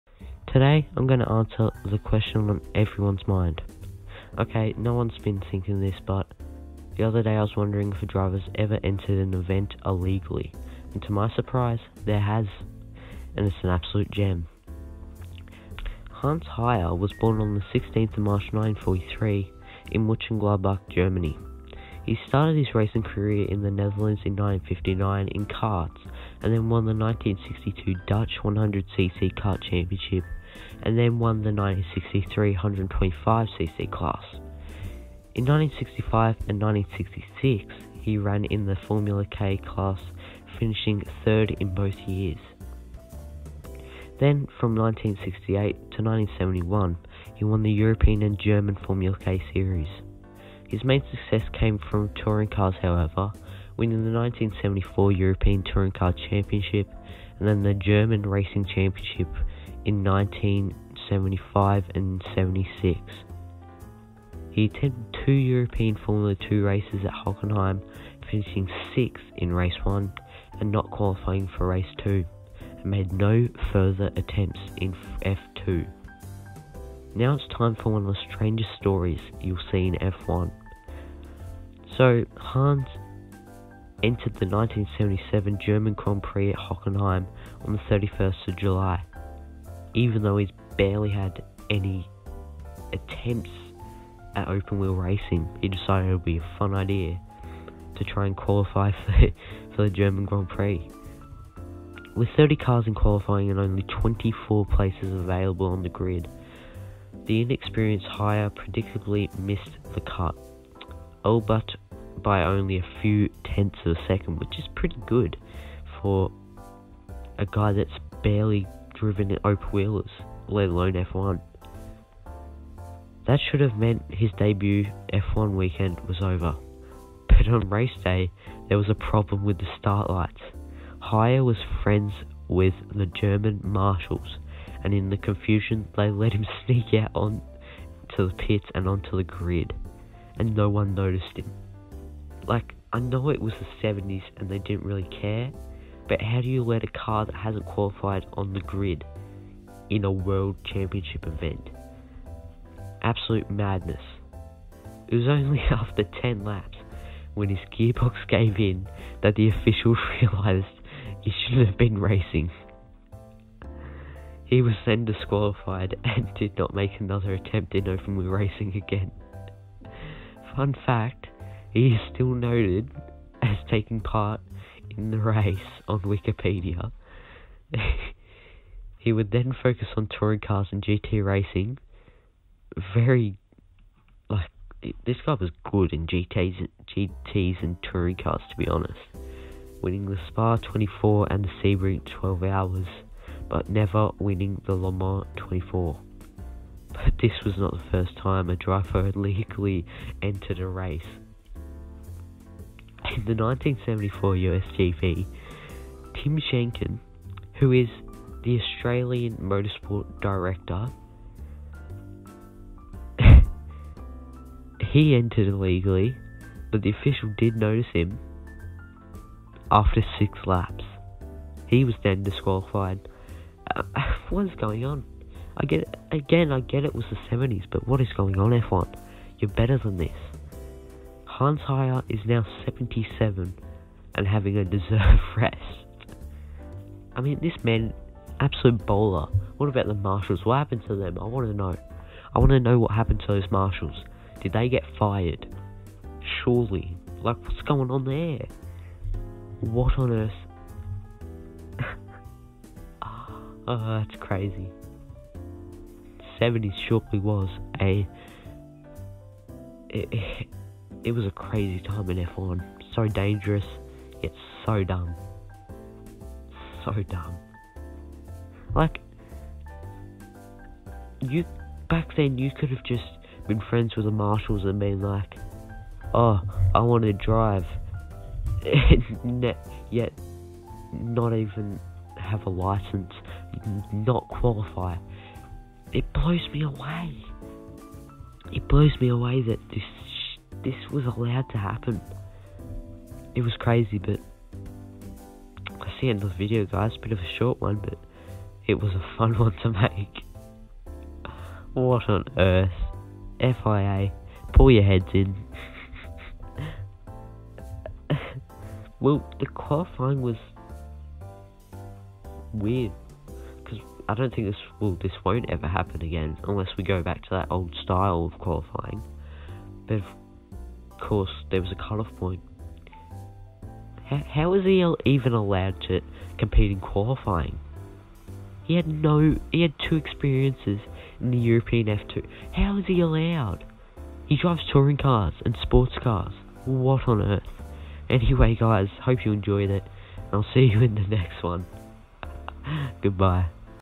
The Driver That Illegally Started A Grand Prix- Racing Tales
The Other Day i was wondering if a driver has ever illegally entered a Formula One Grand Prix and to my surprise there has been at least two. Hans Heyer didn't qualify for the 1977 German Grand Prix but raced anyway.